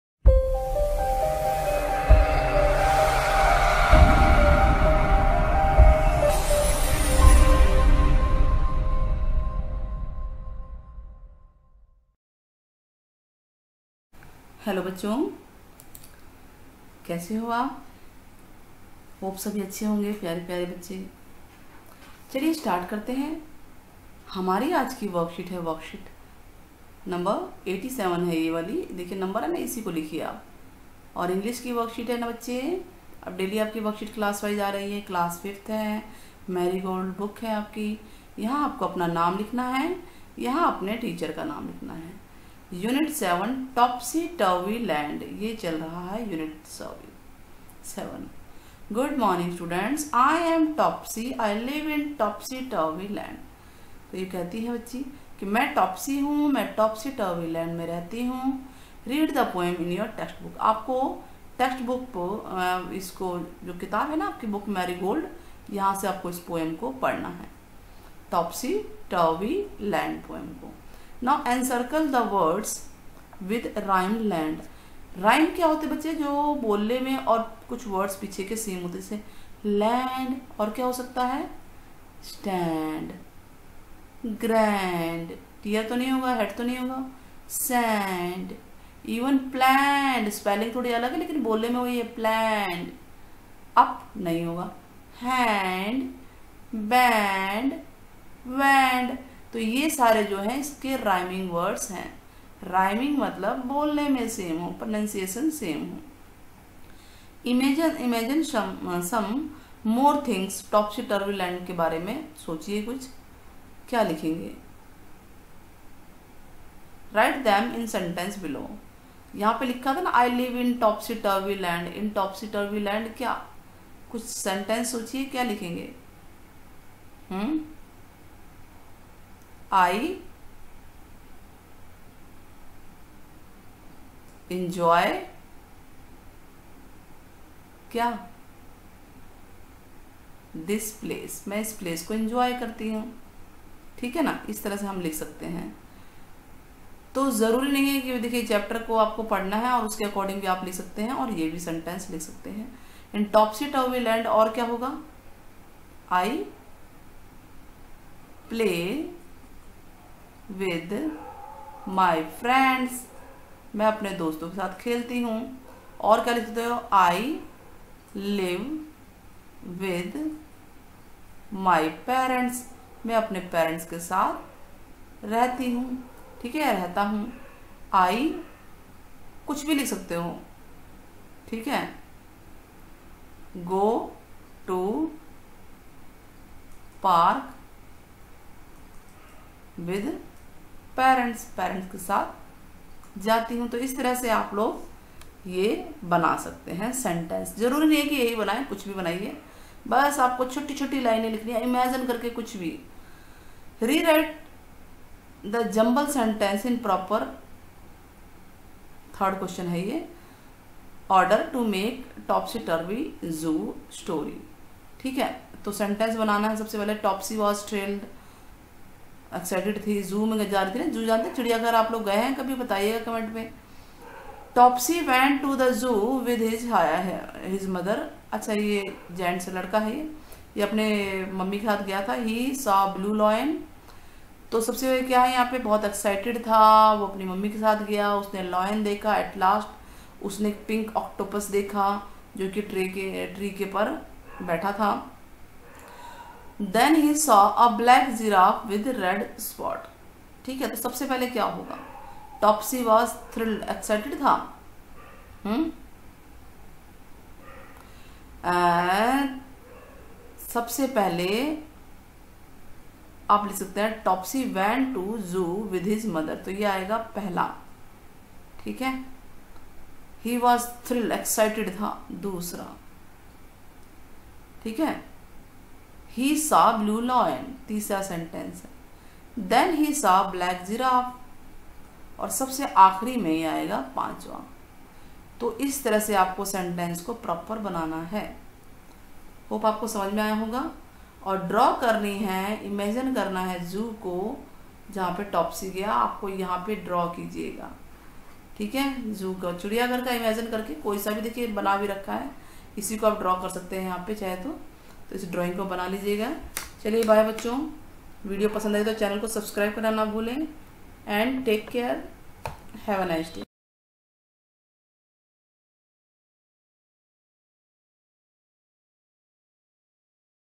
Class 5 Worksheet 87| English | 14 Oct 2020| Edu Villa Point
class 5 || doe work sheet 87|| 14/10/2020 || Directorate of education GNCTD  easy explanation with questions answers hindi
Hindi Worksheet for class5